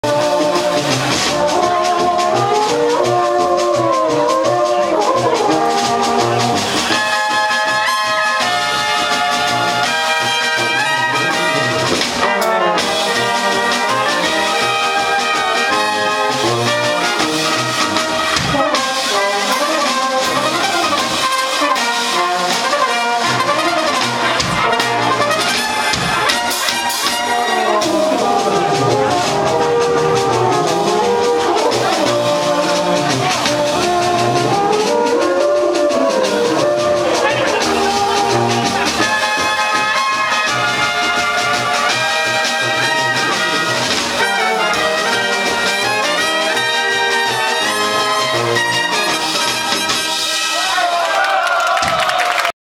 Innsbrucker Böhmische im Vlado Kumpan Stil
Die Innsbrucker Böhmische live am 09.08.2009 in Meran/Obermais (Südtirol).